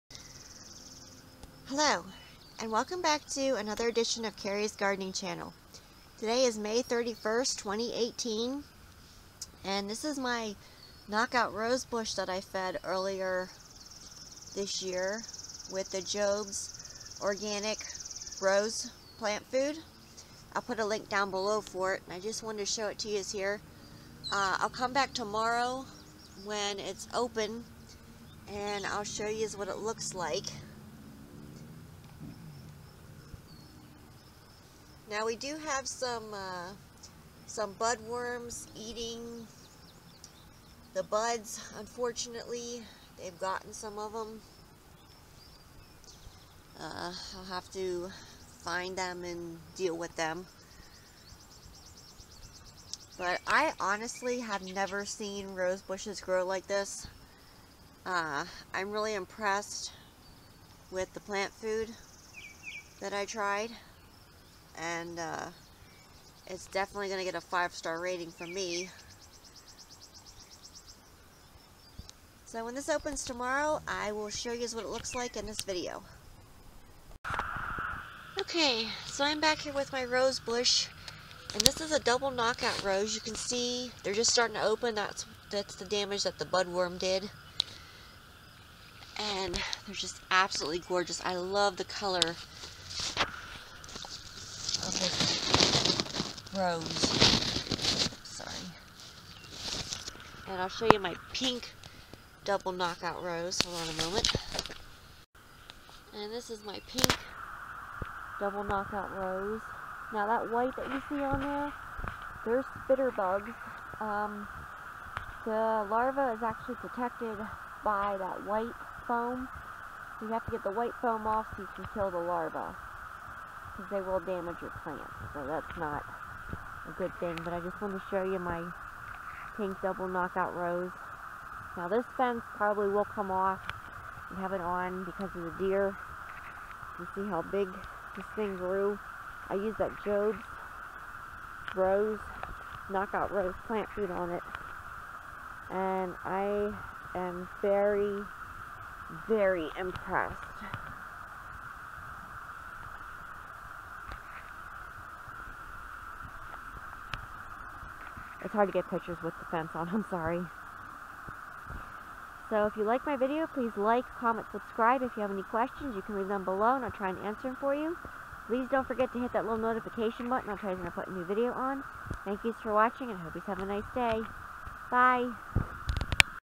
double knock out roses, what do double knock out roses look like, how to feed double knock out roses
In this video I will be showing you my double knock out rose bushes blooming. I put a link further down in the description for the plant food.

Jobes Organic Roses food